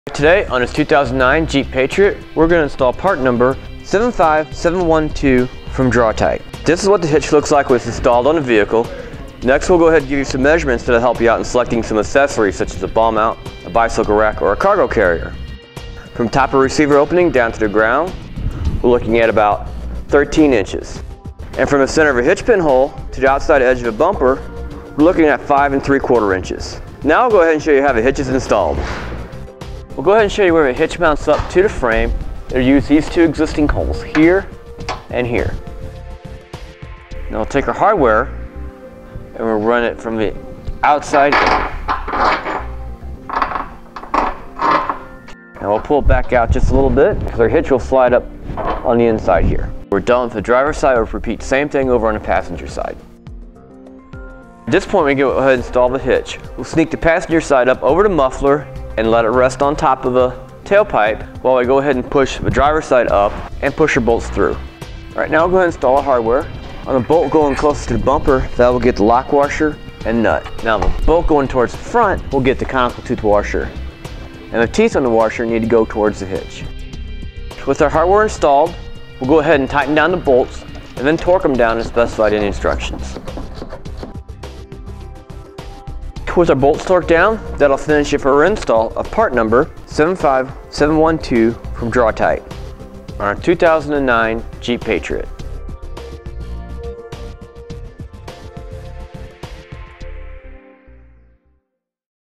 etrailer | DIY 2009 Jeep Patriot Installation for the Draw-Tite Max-Frame Trailer Hitch Receiver
Today on this 2009 Jeep Patriot, we're going to install part #75712 from draw type. This is what the hitch looks like once installed on the vehicle and that's--we'll go ahead and do some measurements that'll help you out in selecting some accessories such as a ball mount, a bicycle rack or a cargo carrier, From top of receiver opening down to the ground, we're looking at about 13 inches. And from the center of a hitch pin hole to the outside edge of the bumper, we're looking at five and three-quarter inches. Now we'll go ahead and show you how the hitch is installed. We'll go ahead and show you where a hitch mounts up to the frame and use these two existing holes here and here. Now we'll take a hardware and we'll run it from the outside and we'll pull back out just a little bit so the hitch will slide up on the inside here.

We're done with the driver side, repeat the same thing over on the passenger side. This point we go ahead install the hitch. We'll sneak the passenger side up over the muffler and let it rest on top of the tailpipe while I go ahead and push the driver side up and push the bolts through. All right, now I'm going to install a hardware on a bolt going close to the bumper that will get the lock washer and nut. Now, when bolt go in towards the front, we'll get the counter tube washer.

And the teeth and the washer need to go towards the hitch. With our hardware installed we'll go ahead and tighten down the bolts and then torque them down as best sliding instructions. With our bolt torque down that will finish you for our install of part #75712 from draw type on a 2009 Jeep Patriot.